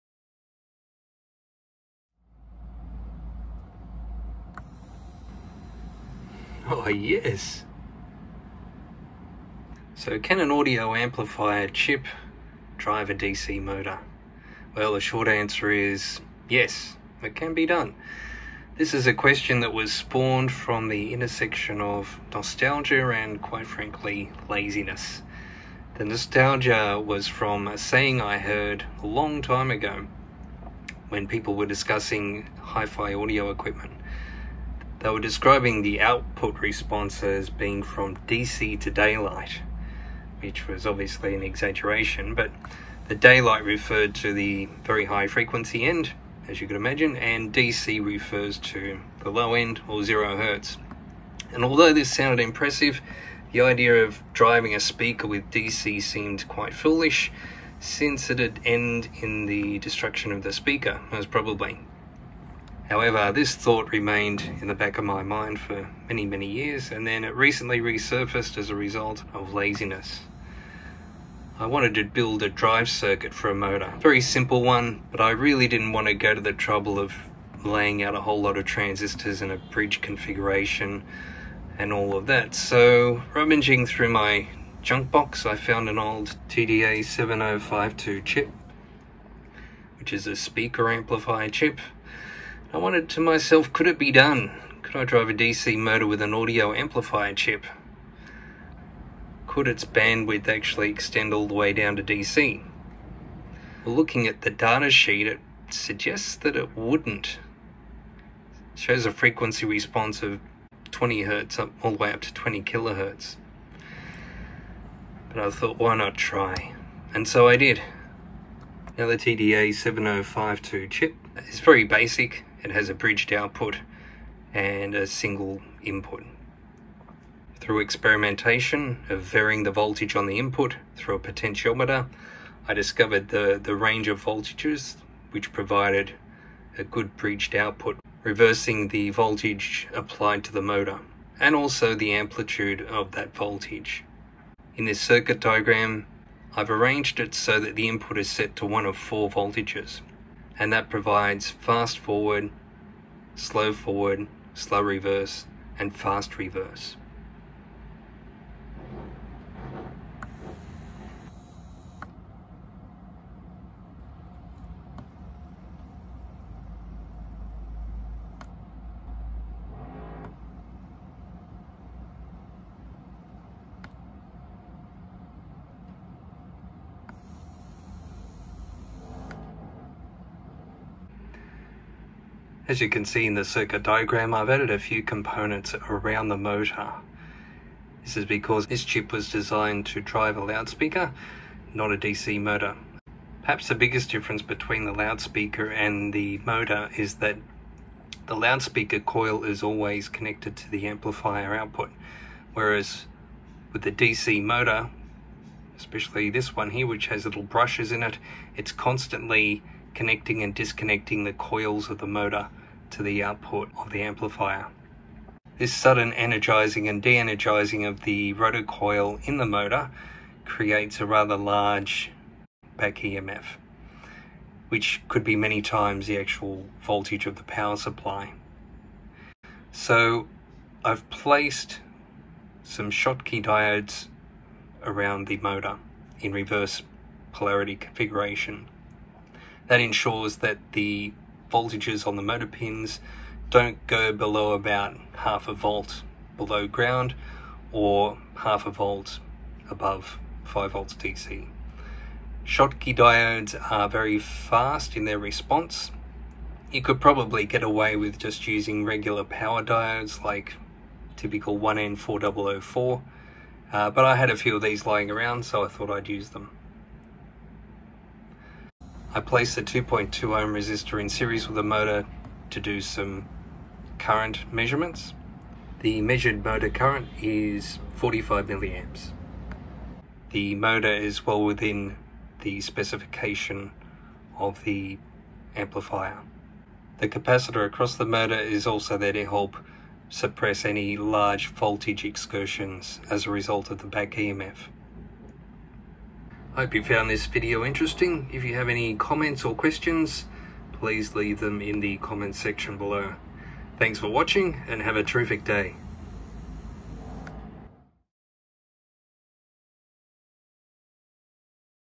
An audio amp can drive a motor?
I was too lazy to make a transistorised bridge circuit to drive a motor. I found an old audio amp chip in my junk box and got to wondering ... could it be done?

Chapters

0:00 Introduction
1:29 TDA7052 Audio Amplifier Chip
1:43 Frequency Response
2:07 Empirically Derived Drive Input Voltages
2:25 Schematic Diagram
2:39 Demonstration of Motor Driver
3:00 Differences Between Driving Motors and Speakers
3:48 Additional Design Considerations